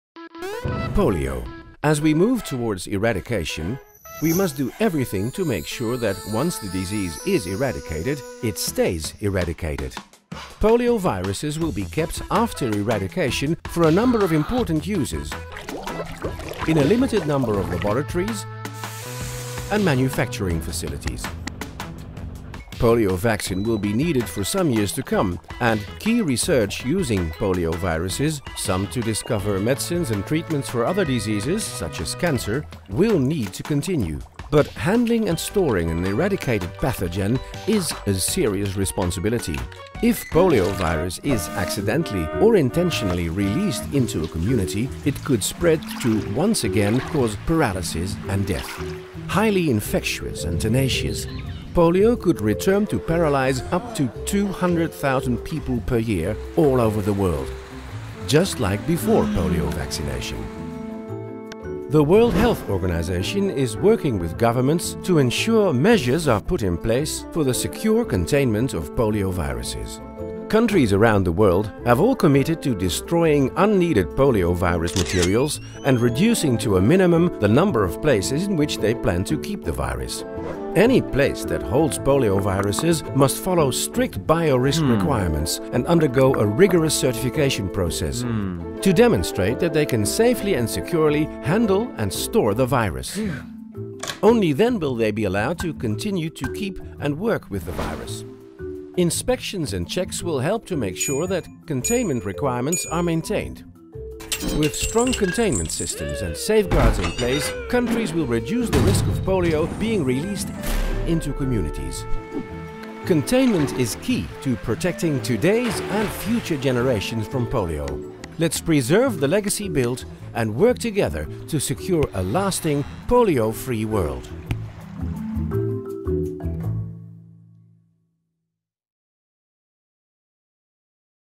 Poliovirus containment animation
Containment of polioviruses is crucial to securing a lasting polio-free world. Polioviruses will be kept after eradication in a limited number of laboratories and vaccine manufacturing facilities worldwide. Containment of these viruses is about making sure that safeguards are in place to minimize the risk of their release into communities, which could have devastating consequences.